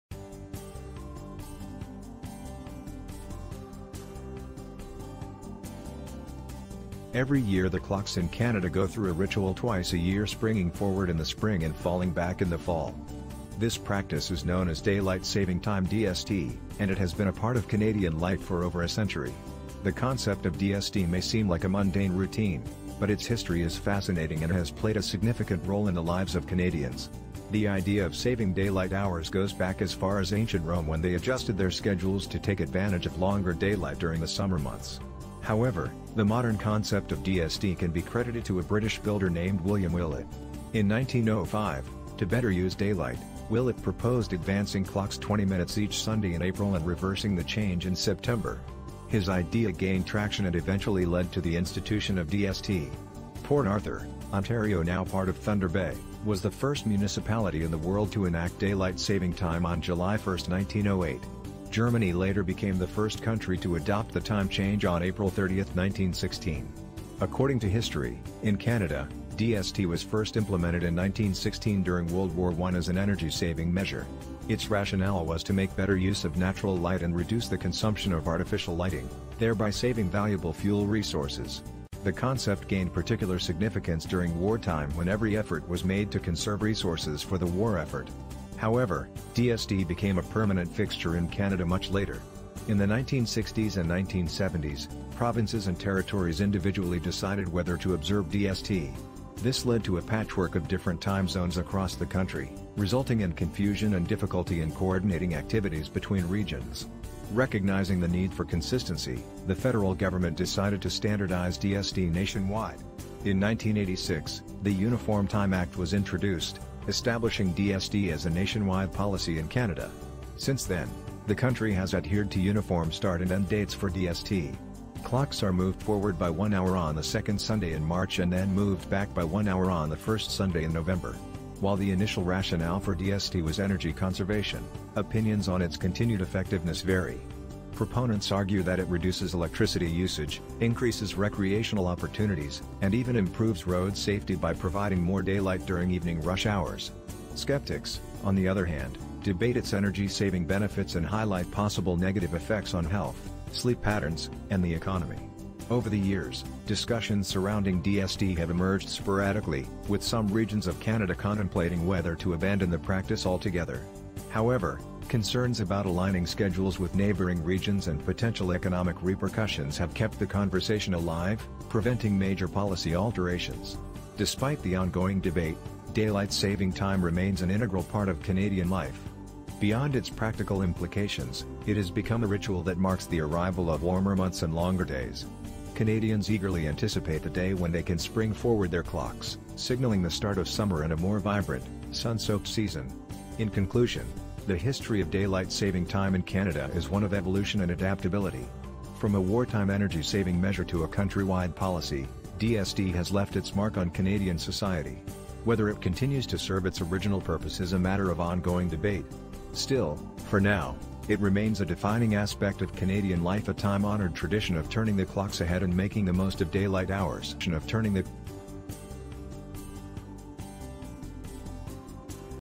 Do Canadians Benefit from Canada Daylight Saving Time
Canada Daylight Saving Time: Every year the clocks in Canada go through a ritual twice a year – springing forward in the spring and falling back in the fall. This practice is known as Daylight Saving Time (DST), and it has been a part of Canadian life for over a century. The concept of DST may seem like a mundane routine, but its history is fascinating and has played a significant role in the lives of Canadians.

The idea of saving daylight hours goes back as far as Ancient Rome when they adjusted their schedules to take advantage of longer daylight during the summer months. However, the modern concept of DST can be credited to a British builder named William Willett. In 1905, to better use daylight, Willett proposed advancing clocks 20 minutes each Sunday in April and reversing the change in September. His idea gained traction and eventually led to the institution of DST.

Port Arthur, Ontario (now part of Thunder Bay), was the first municipality in the world to enact daylight saving time on July 1, 1908. (Germany later became the first country to adopt the time change on April 30, 1916.)

According to history, in Canada, DST was first implemented in 1916 during World War I as an energy-saving measure. Its rationale was to make better use of natural light and reduce the consumption of artificial lighting, thereby saving valuable fuel resources. The concept gained particular significance during wartime when every effort was made to conserve resources for the war effort.

However, DST became a permanent fixture in Canada much later. In the 1960s and 1970s, provinces and territories individually decided whether to observe DST. This led to a patchwork of different time zones across the country, resulting in confusion and difficulty in coordinating activities between regions. Recognizing the need for consistency, the federal government decided to standardize DST nationwide.

In 1986, the Uniform Time Act was introduced, establishing DST as a nationwide policy in Canada. Since then, the country has adhered to uniform start and end dates for DST. Clocks are moved forward by one hour on the second Sunday in March and then moved back by one hour on the first Sunday in November.

While the initial rationale for DST was energy conservation, opinions on its continued effectiveness vary. Proponents argue that it reduces electricity usage, increases recreational opportunities, and even improves road safety by providing more daylight during evening rush hours. Skeptics, on the other hand, debate its energy-saving benefits and highlight possible negative effects on health, sleep patterns, and the economy.

Over the years, discussions surrounding DST have emerged sporadically, with some regions of Canada contemplating whether to abandon the practice altogether. However, concerns about aligning schedules with neighbouring regions and potential economic repercussions have kept the conversation alive, preventing major policy alterations.

Despite the ongoing debate, Daylight Saving Time remains an integral part of Canadian life. Beyond its practical implications, it has become a ritual that marks the arrival of warmer months and longer days. Canadians eagerly anticipate the day when they can spring forward their clocks, signalling the start of summer and a more vibrant, sun-soaked season.

While there are several advantages to DST, there are also significant drawbacks that have led many individuals to question the necessity of this time adjustment. One prominent disadvantage revolves around the disruption to sleep schedules. Changing the clocks by one hour exaggerates the already widespread problem of sleep deprivation. Many people find it challenging to adapt to the new time immediately, resulting in adjustment difficulties, fatigue, and reduced productivity.

Furthermore, the implementation of DST poses inconveniences in modern society. Changing the time twice a year can cause confusion and disrupt schedules, particularly for those living in close proximity to different time zones or those who have business dealings across borders. This can lead to missed appointments, delays, and inefficiencies in various sectors, including transportation, commerce, and communication.

Moreover, critics argue that the energy conservation benefits claimed by DST advocates are not as significant as once believed. As societies evolve, energy usage patterns change and the original reasons for implementing DST may be less relevant in the modern era. Contrary to the anticipated savings, some studies even suggest that DST can lead to increased energy usage in certain situations, such as the increased demand for air conditioning during longer and hotter evenings.